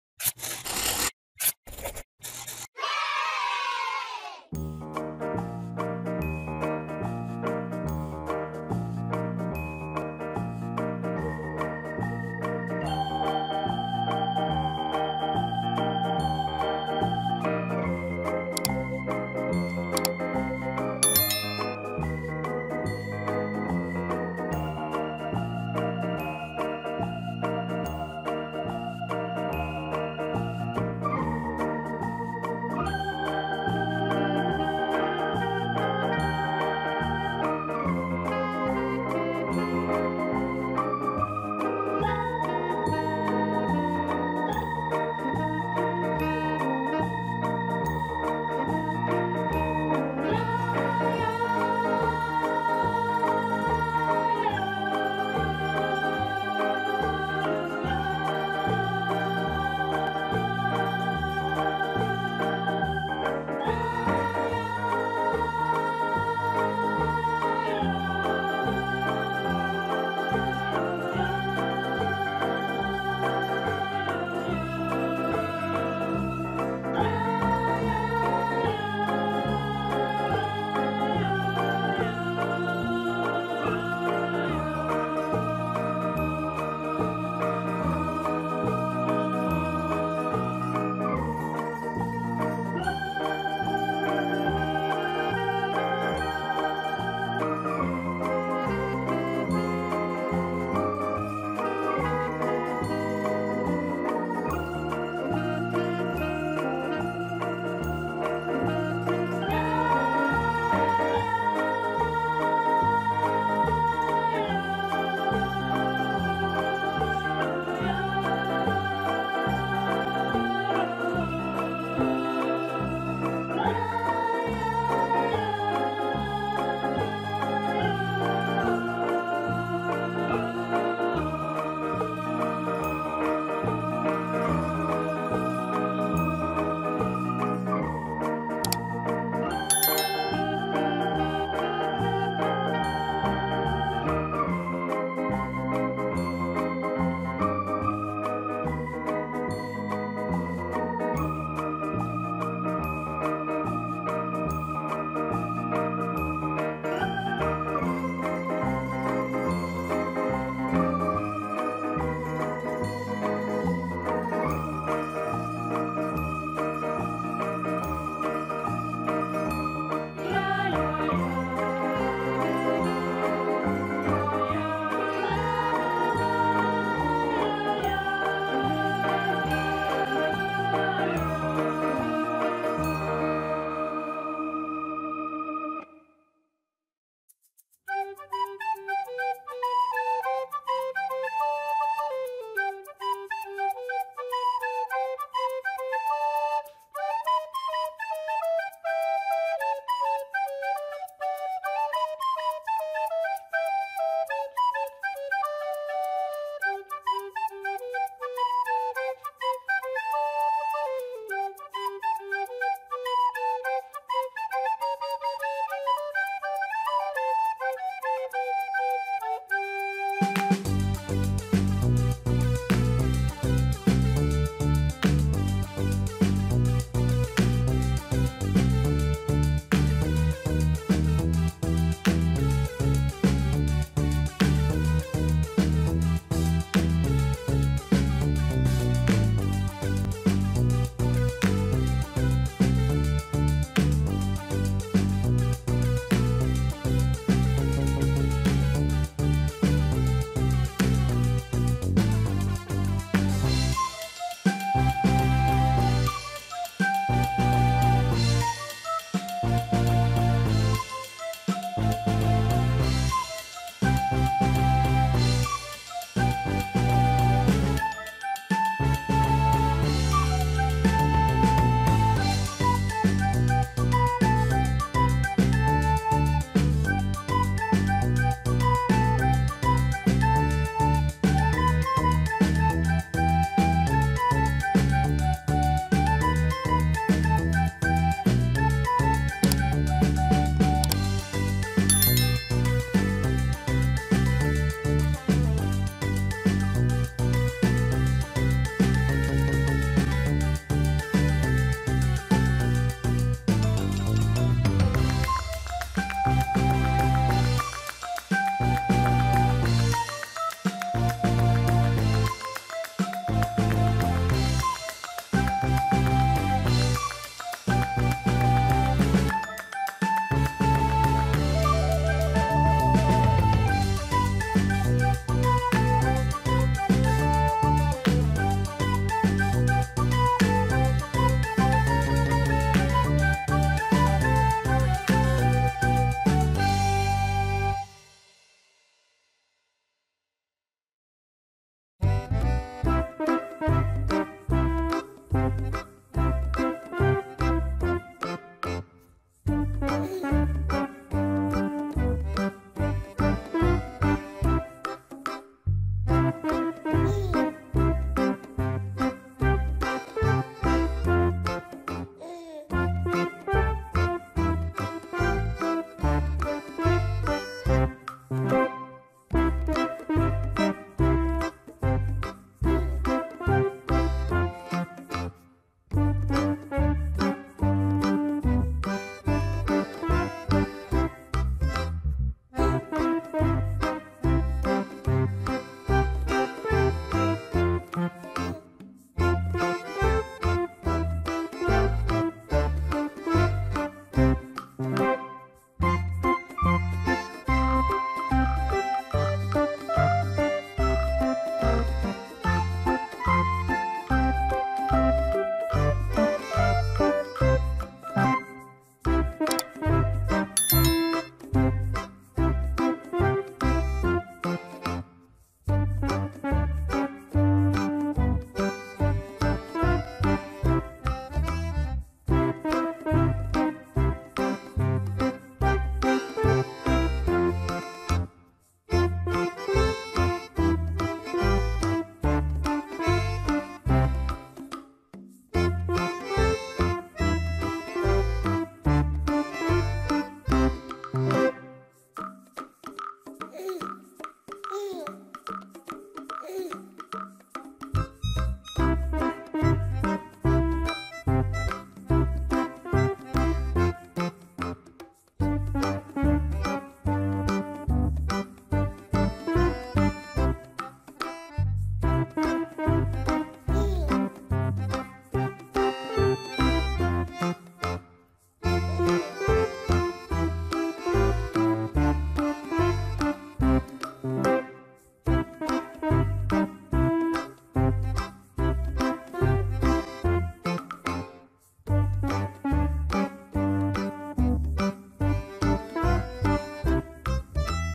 Chicken 20 Stickers Drawing art New 20 Chicken Sticker Emojis Drawing Art
Drawing In This Video:
001-beer
002-bomb detonation
003-surpise
004-cauldron
005-birthday
006-music
007-happy
008-broken heart
009-idea
010-in love
011-surprise
012-mind blown
013-investment
014-music
015-question
016-rainbow
017-read
018-rich
019-sick
020-shock
021-waving
022-workout